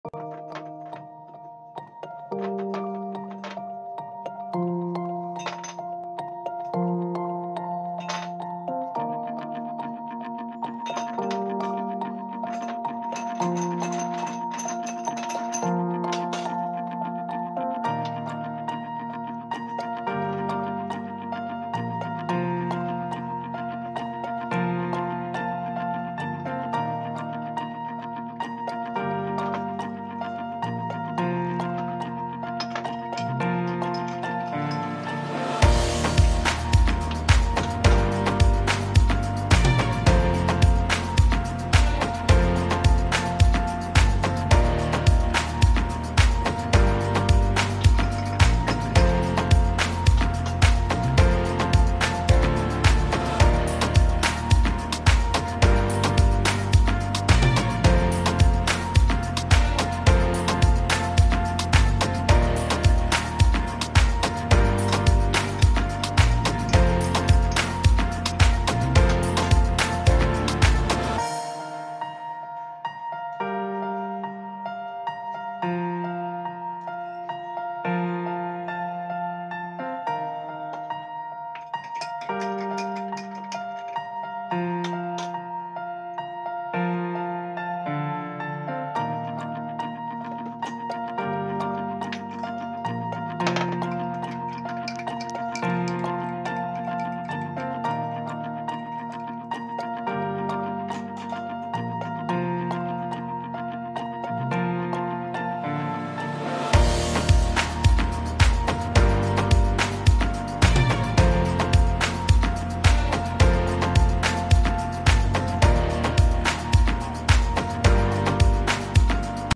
Drink for Good Digestion
Ice Cubes
Water
Chat Masala
Carrom seeds - Ajwain
Cumin Seeds - Zeera